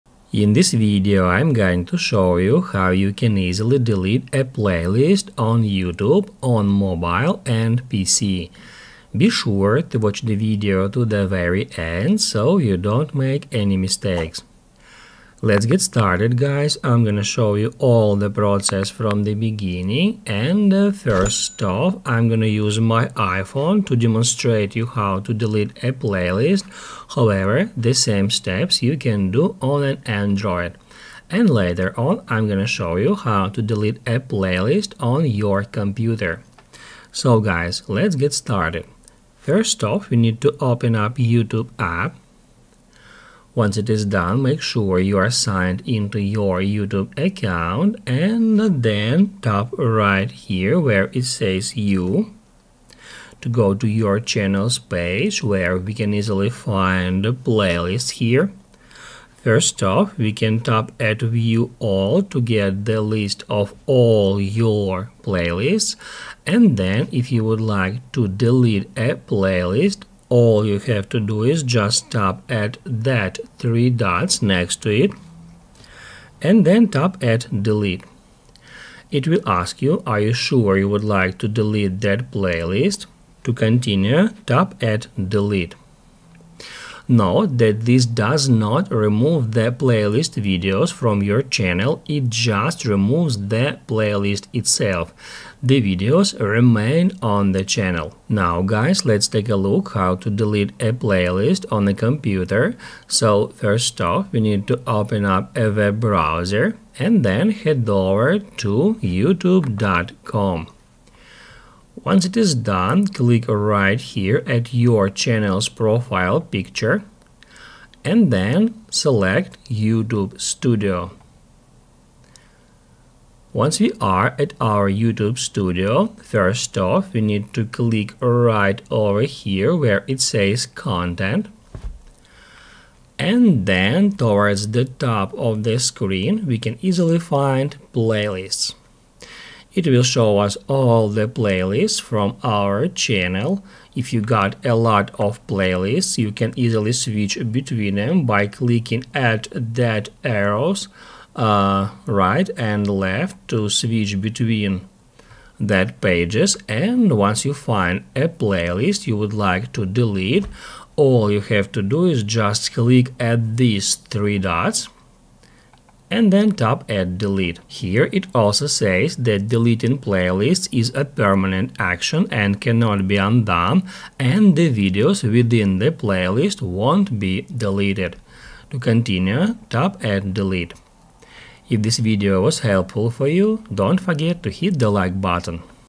How to Delete Playlist on Youtube (Mobile & Pc) - [IPhone 15 Pro]
If you are looking for a video about how to delete playlist on youtube, here it is! In this video I will show you how to delete youtube playlist. Be sure to watch the video to the very end. You will learn how to delete a playlist on youtube. That's easy and simple to do from your phone.